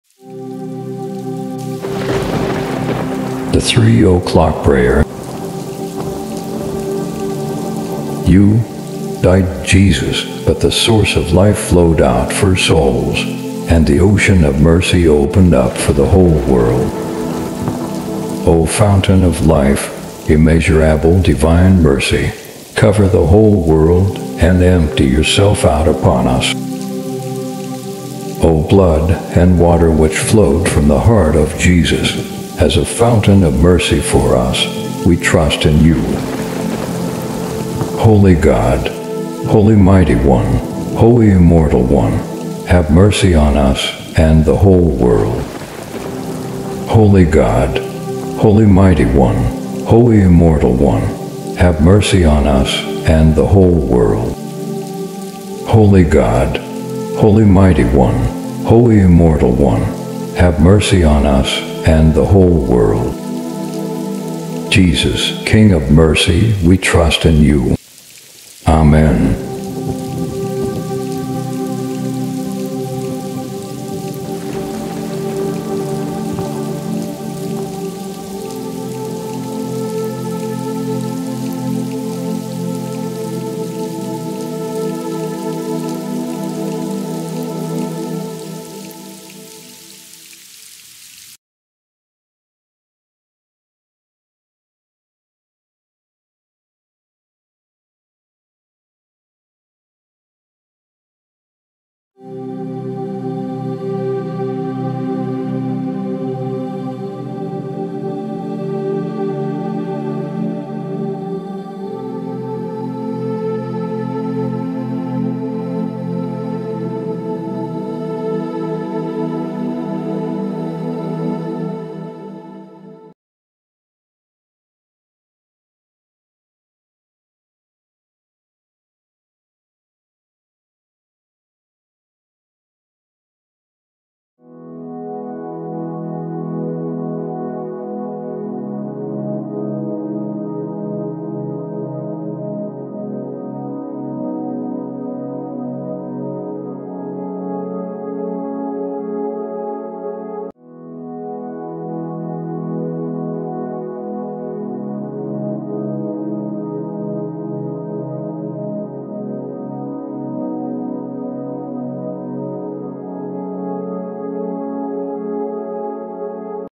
3 o'clock Prayer
In October 1937, in Cracow (Poland), Lord Jesus instructed Sister Faustina to venerate the hour of His death and to connect with Him in prayer referring to the value and merits of His Passion.